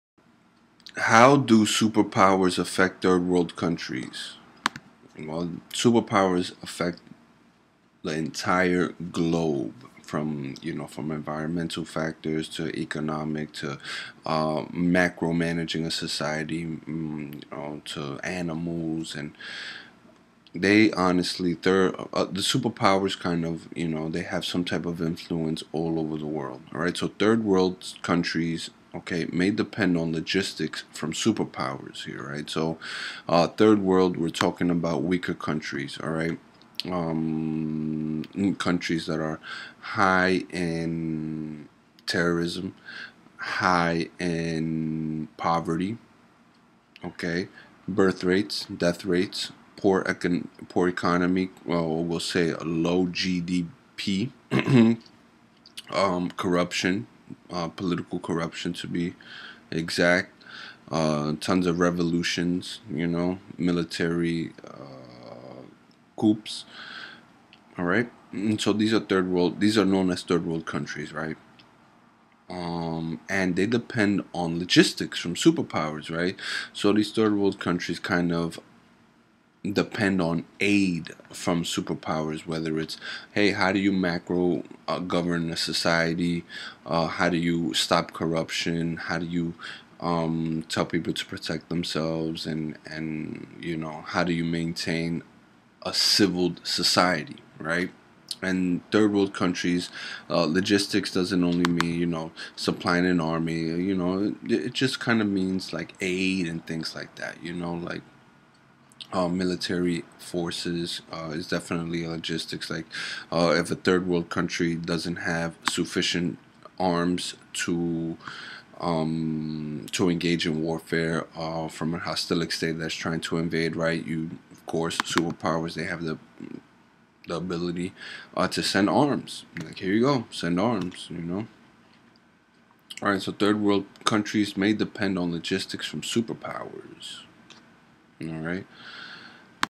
Political Science A27 (13/20) Logistics from superpowers
Third world countries may depend on logistics from superpowers.